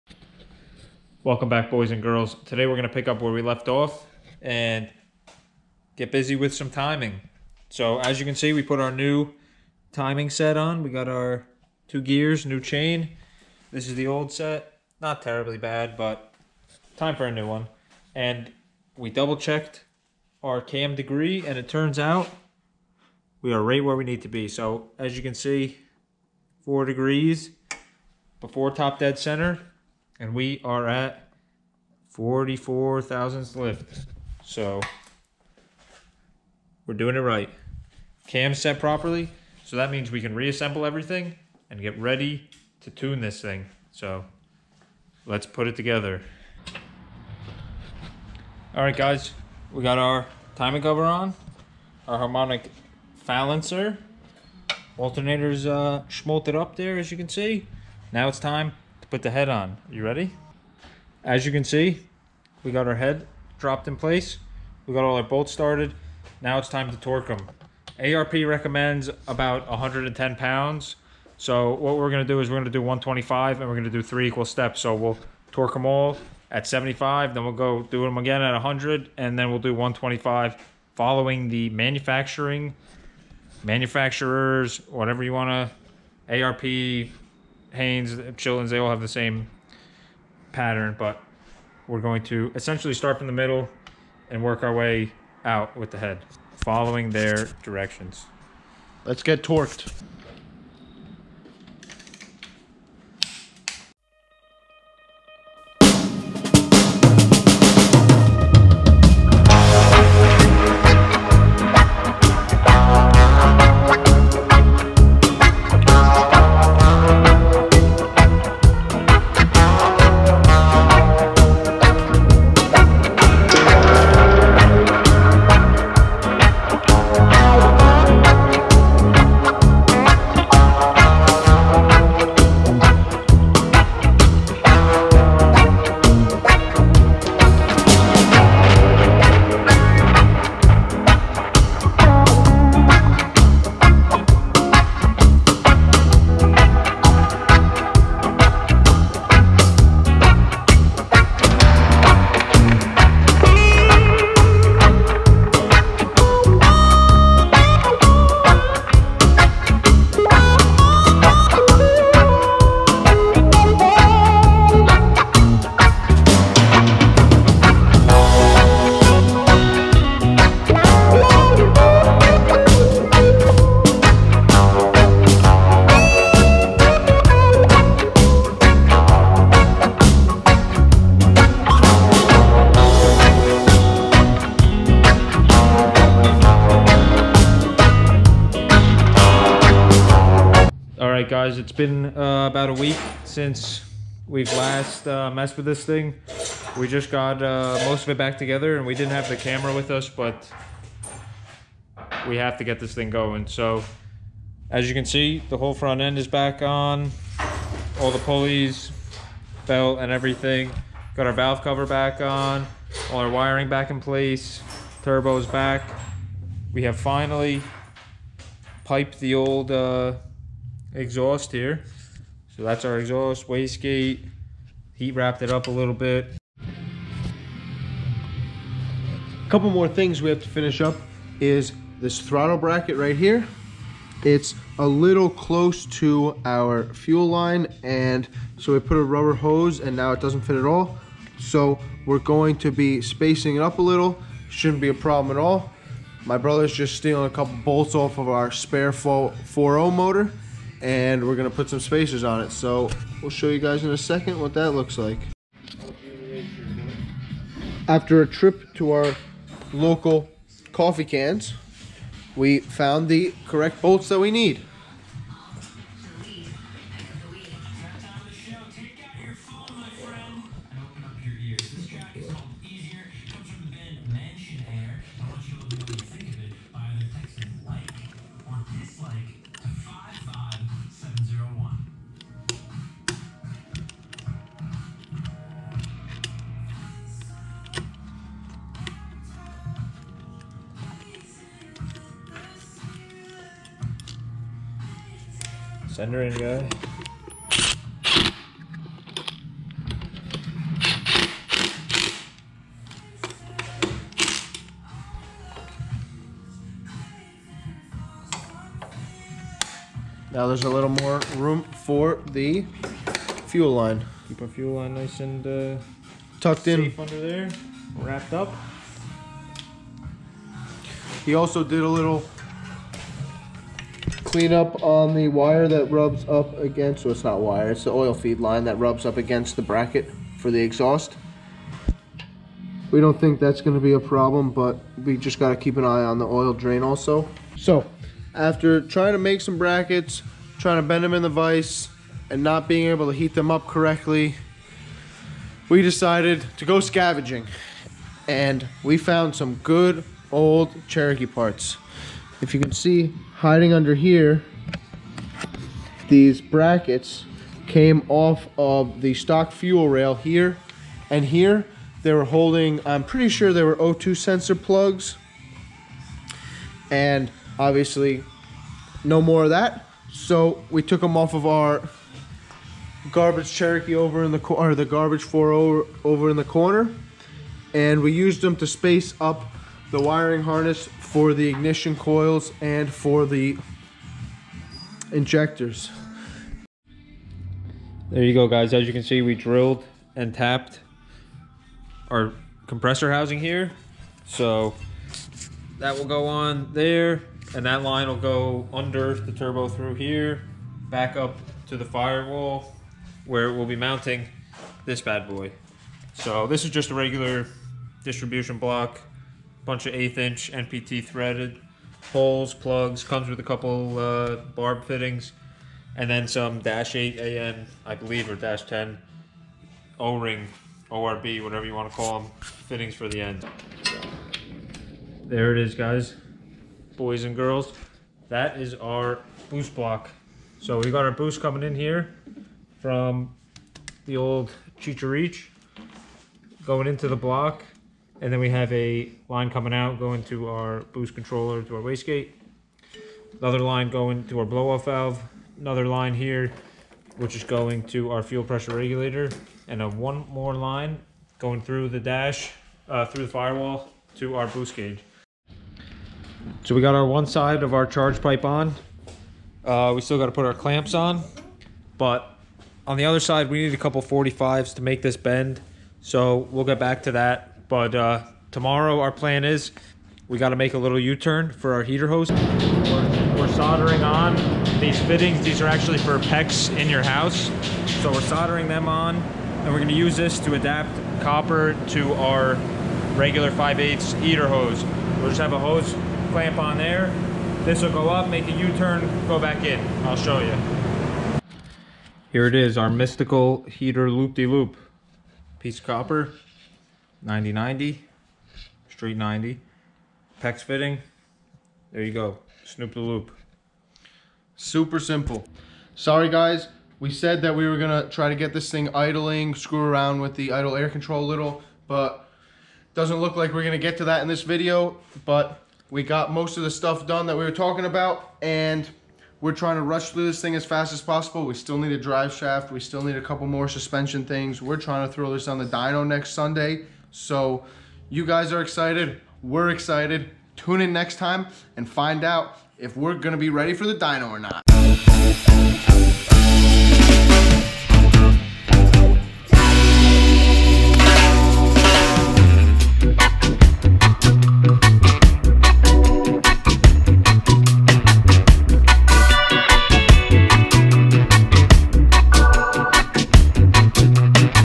Turbo Jeep Cherokee Dyno Prep Part 1
We have just about finished our megasquirt install and double checked our cam degree.  Now we have to get the rest of the turbo system back together and get this dumpster ready to be tuned on the dyno!

Jeff- @jeff_the_heff
Matt- @flipboard7431
Channel- @gruesomegarage

Parts List: So Far
-Megasquirt ms3x
-Fuel Rail
-Painless Performance Universal Fuse Blocks #30004
-80 pound EV1 injectors
-LS2 truck style coils
-GT35 emusa
-Aremotive 340lph stealth 20 gallon cell

Engine: Nothing Special
-ARP head studs
-Lunati cam
-mls head gasket
-LS valve springs